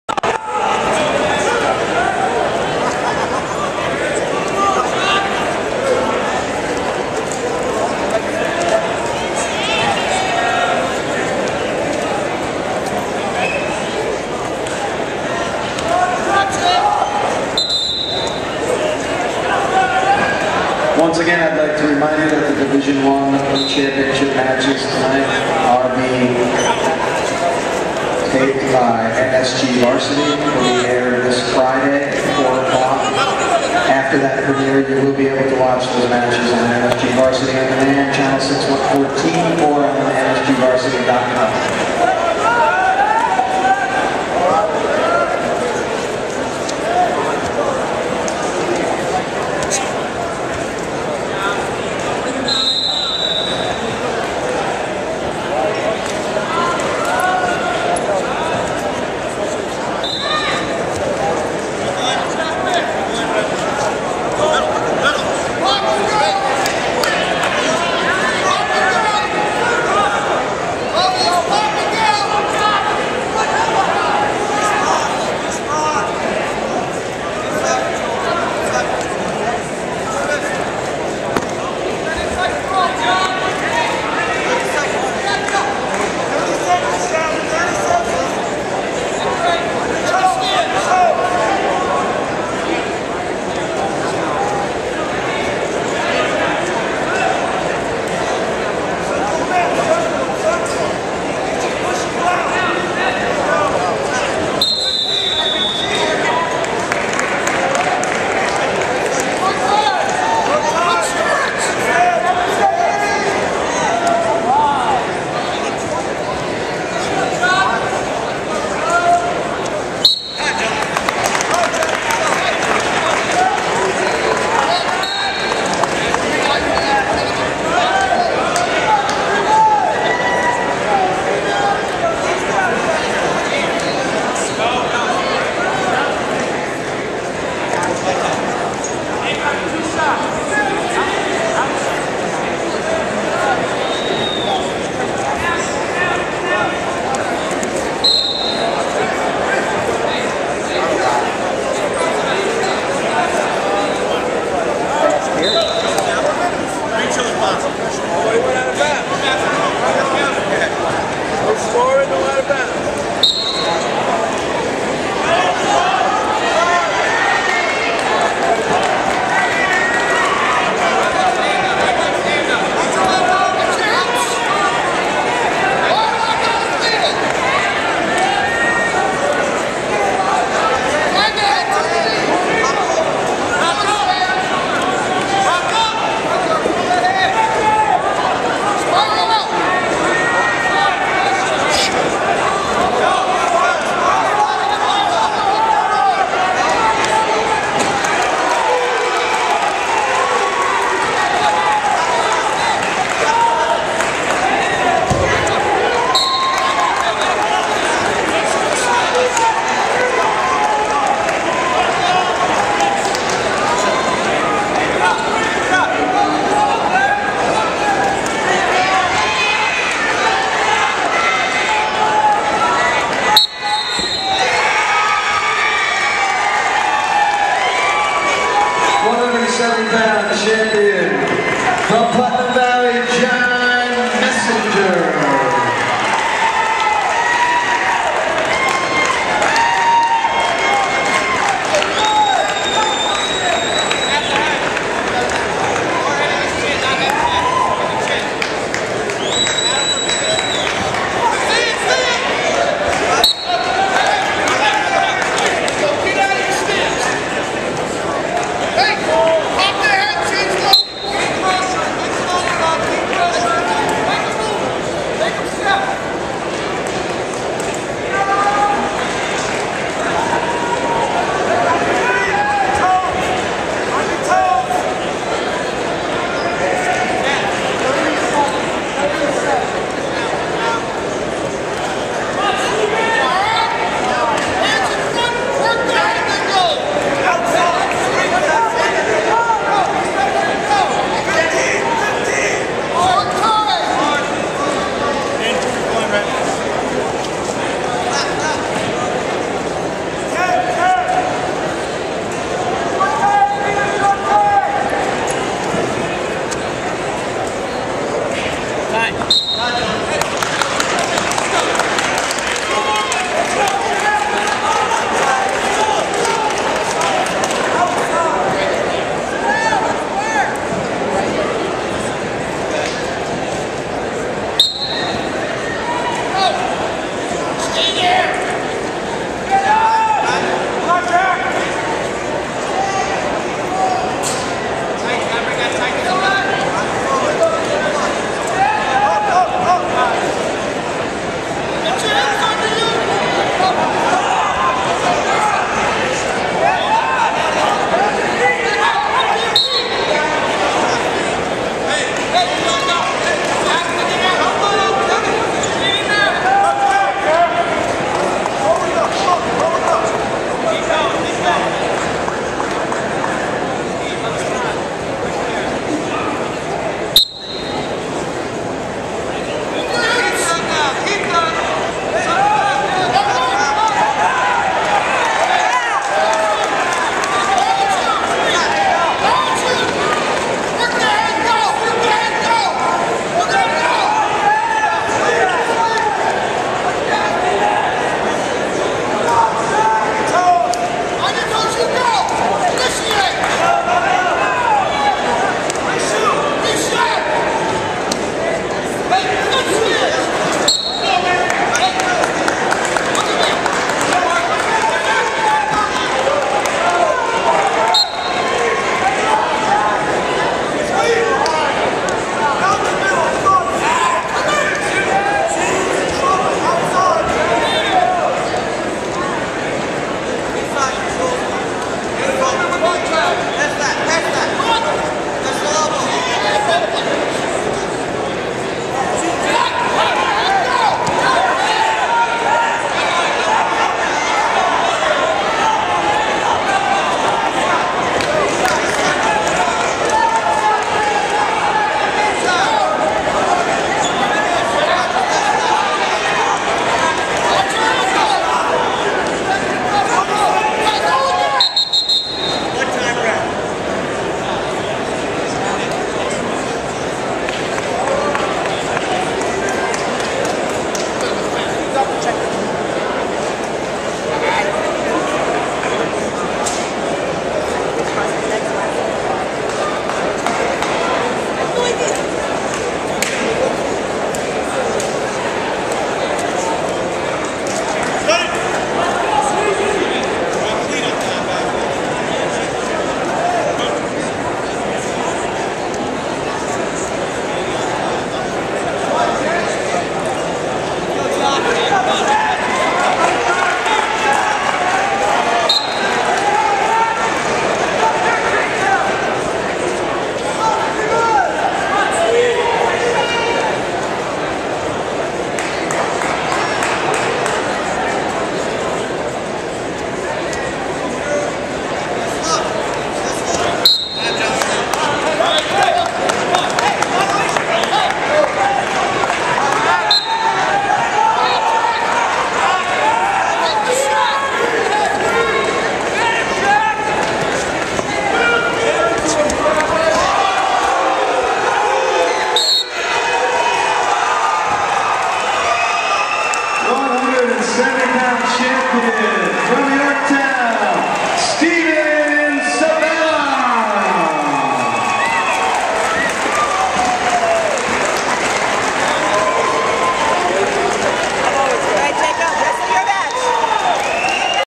Section 1 finals: Steven Sabella (Yorktown) defeats Jack Braun (Scarsdale) in OT
In the 170-pound Division 1 final at the Section 1 championships on 02/12/12 at Pace University, No. 5 Steven Sabella (Yorktown) defeated No. 2 Jack Braun (Scarsdale) in overtime by decision, 4-2.